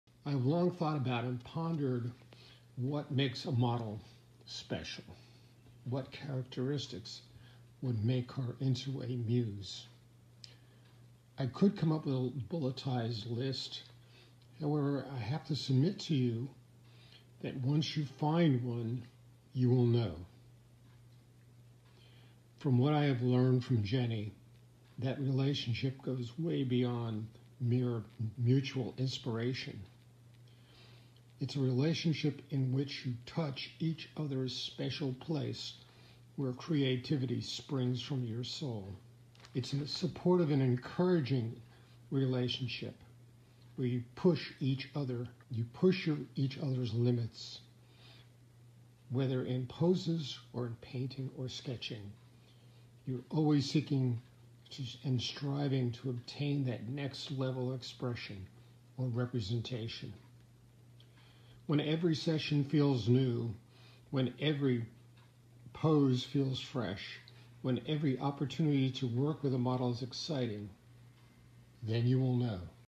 Reflecting on the Muse
Artist Rich Klein talks about the Muse influence